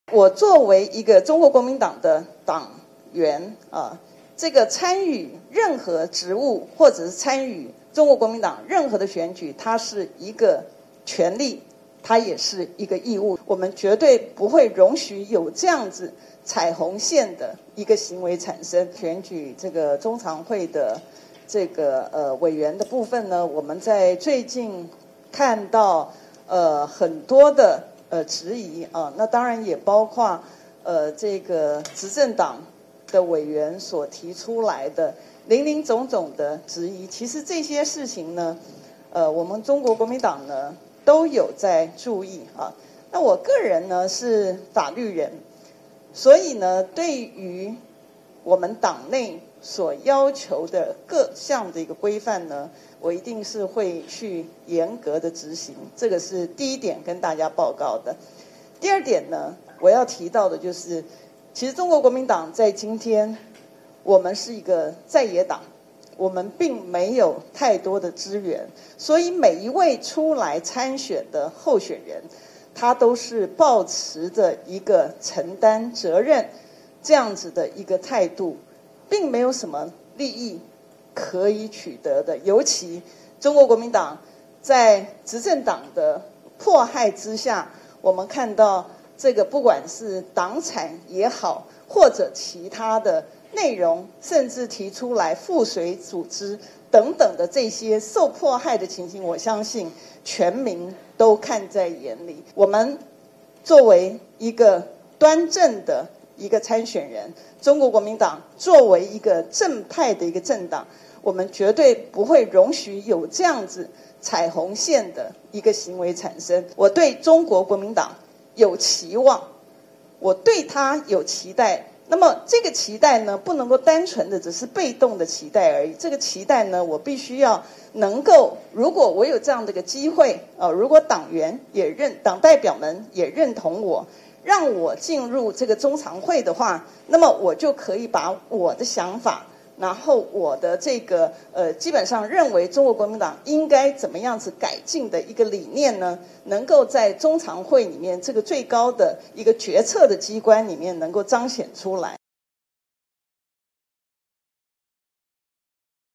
國民黨中常委選風遭疑 李貴敏：法律沒禁止我選
中央委員史碩仁今(21)日舉行記者會盼端正中央委員、中常委「換票」選風。國民黨考紀會主委李貴敏今回應，盼參選人自律，並提供檢舉專線和信箱，強調國民黨不容許有踩紅線情況發生。但對於自己身為黨務主管也參選中常委，她則說，法律沒禁止她選，希望自己參與中常會，讓國民黨更好。